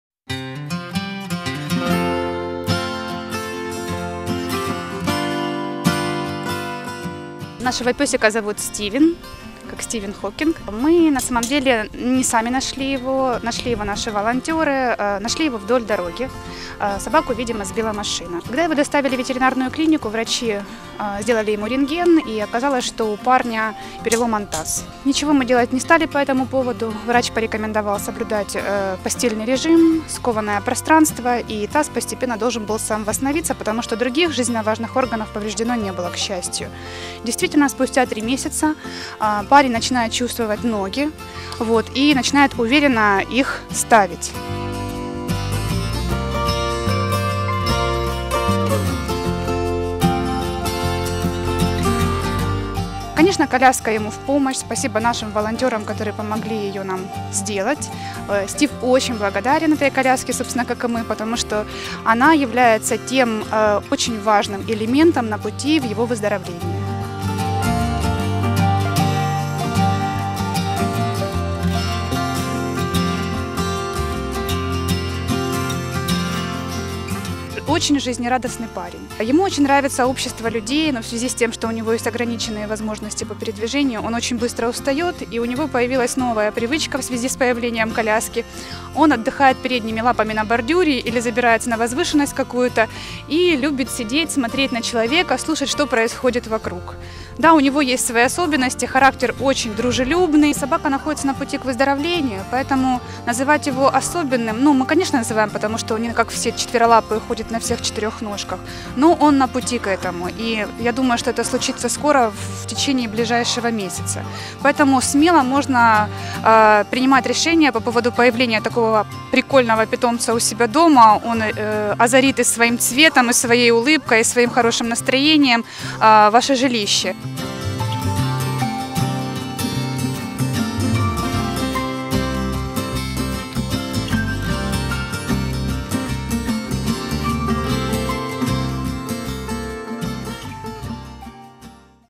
История одной трогательной собаки в Фельдман Экопарк
Стивен мог бы затмить само солнце - настолько в нем много энергии, добра и теплоты! Но ему не повезло с людьми – его сбили автомобилем и бросили покалеченным у дороги. Вовремя малыш попал на глаза волонтерам, которые тут же привезли животное к специалистам. Операция по спасению Стивена и подробности того, как он научился вновь доверять людям в нашем репортаже!

Story about one touching dog in Feldman Ecopark

Stephen could eclipse the sun – this dog has so much energy, kindness and warmth! But it had bad luck with people: it was knocked down by a car and left disabled near the road. Luckily, the kid was found by the volunteers, who immediately brought the animal to the specialists. Our new video is about Stephen’s rescue operation and the details of how the dog started to trust people again!